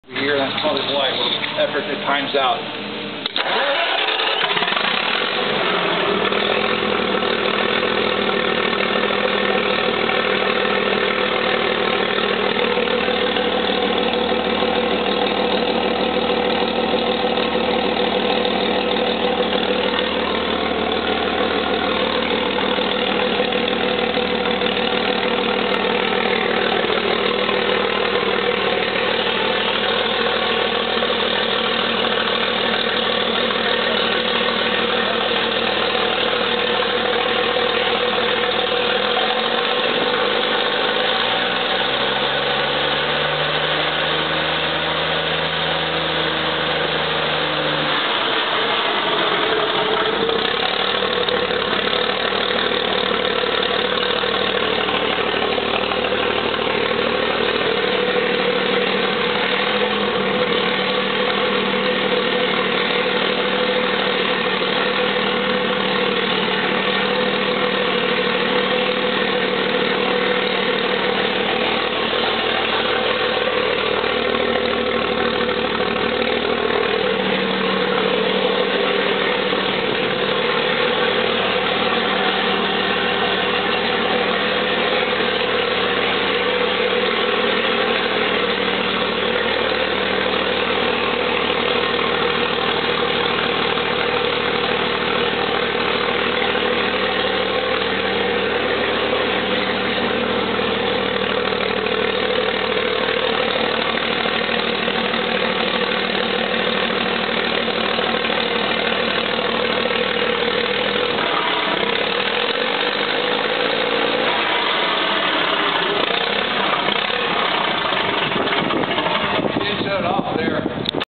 Cummins G855 Power Unit Stock 55299
SOLD:  Complete natural gas power unit on stand radiator through clutch, Mfg. 10/06.